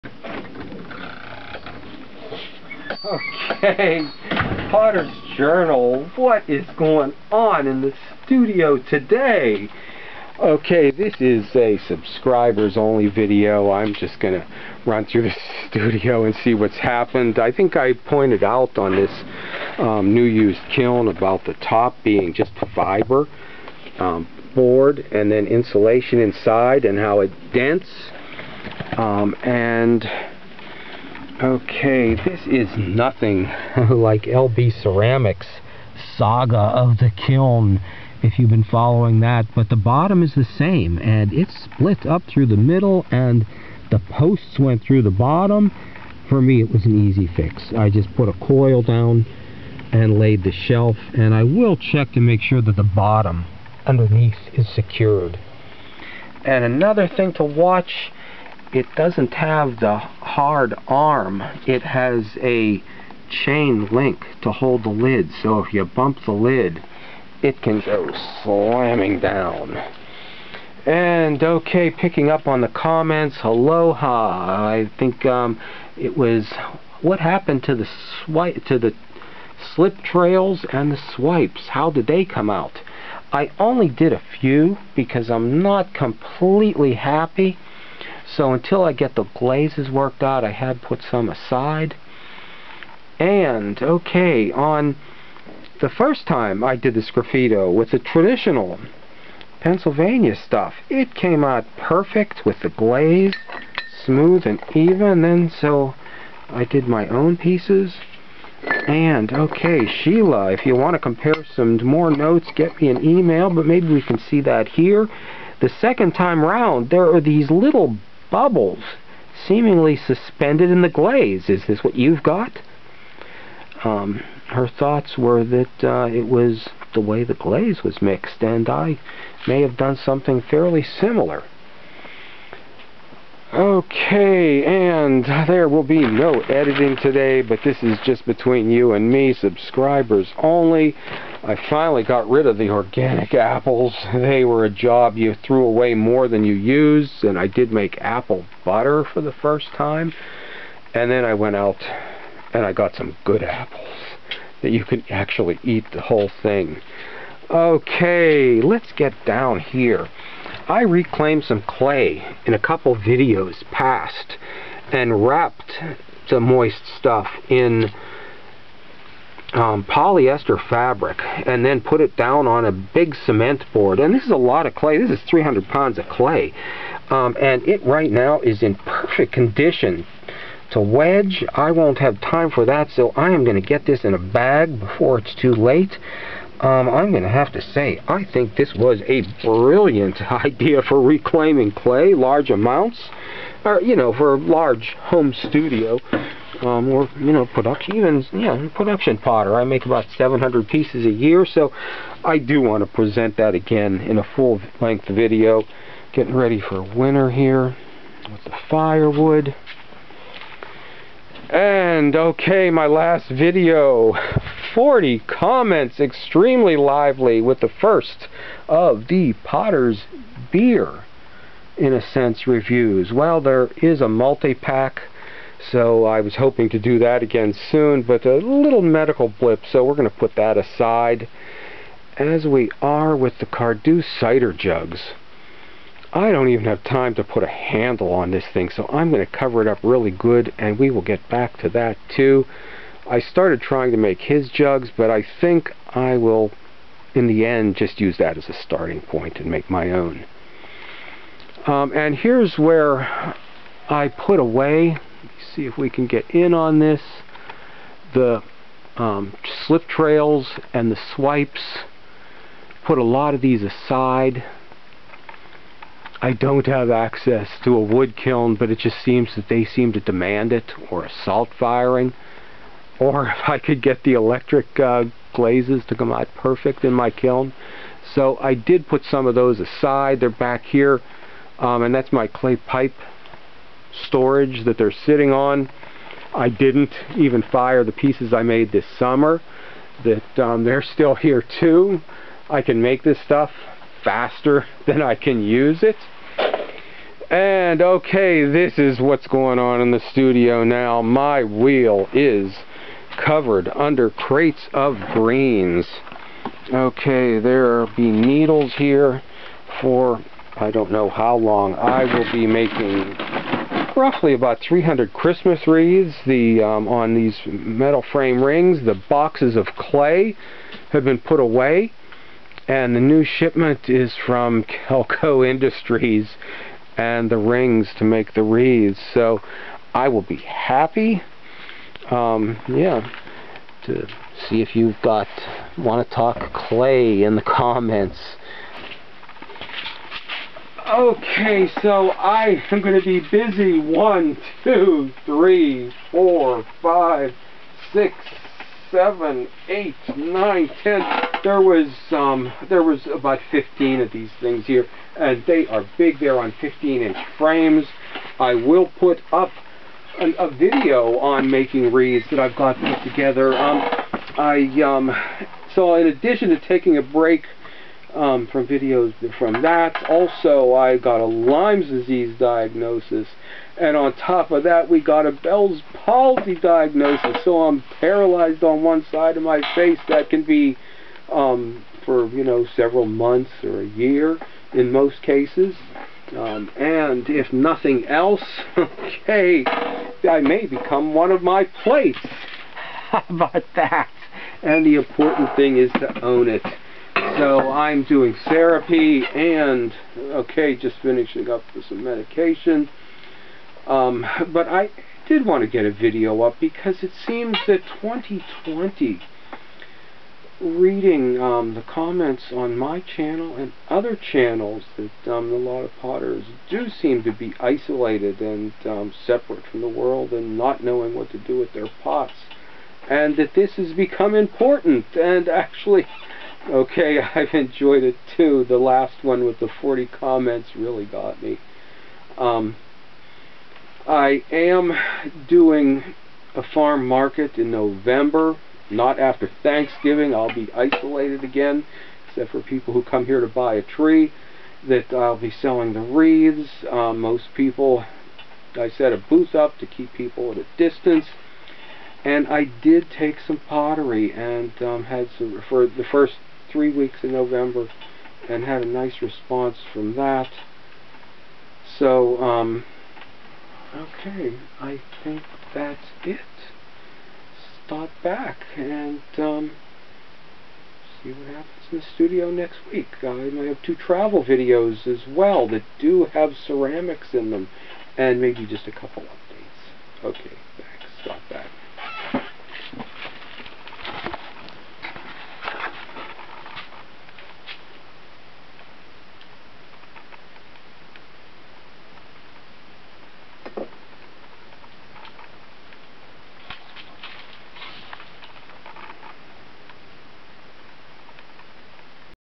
Subscribers only, POTTERY STUDIO VLOG, november 2020 #341 potters journal
The year winds down and what a year it's been. For me a time to get in touch with what is real and what is important. It's been a gift to enjoy my farm, smell the roses, touch the earth, work the clay and think about my art. Through it all it's been satisfying to know that what I've done has helped other navagate this year. Now as the last 6 weeks have turned on me the 'community' and 'dialog' of your 'comments' has sustained me.Thanks
Jeff Kohut, potters jornal, November 2020.